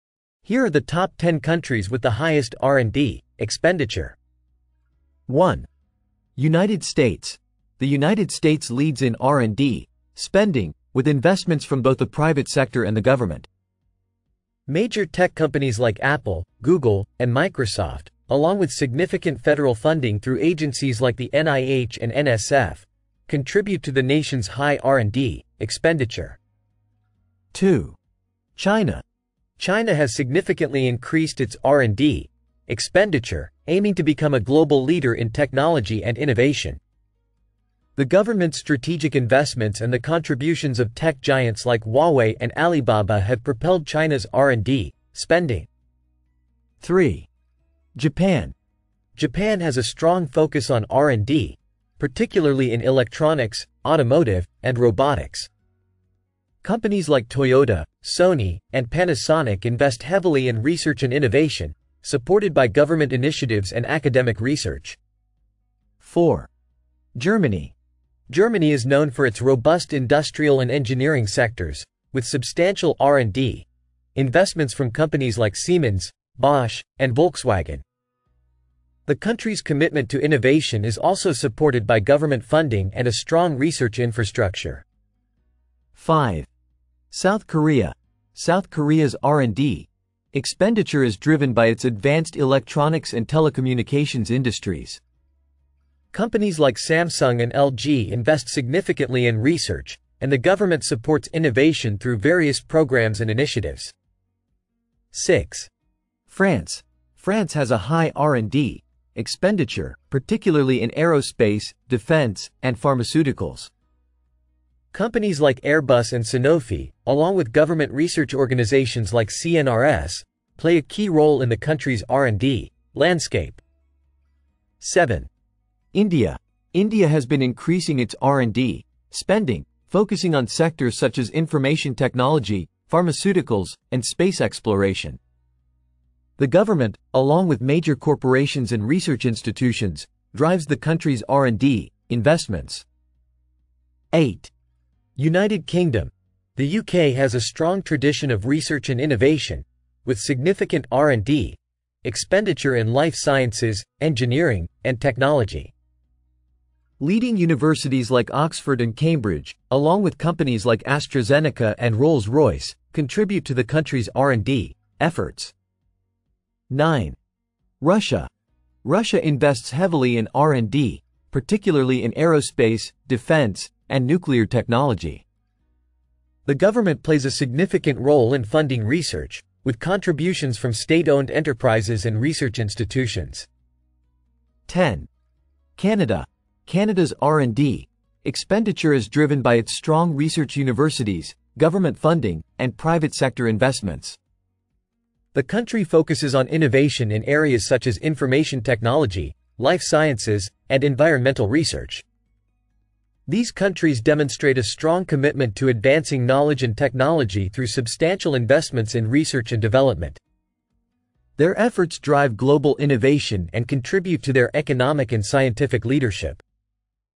Top 10 Countries with the Highest R&D Expenditure
Explore the top 10 countries with the highest research and development (R&D) expenditure. See where the most significant investments in innovation and technology are made.